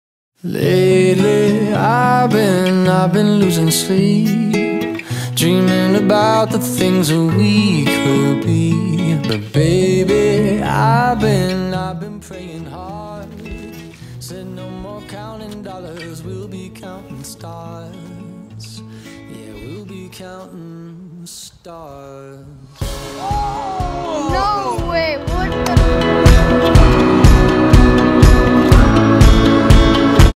My Coolest Birdie Ever
Skip shot on Hole 4 at Auburn caught on camera!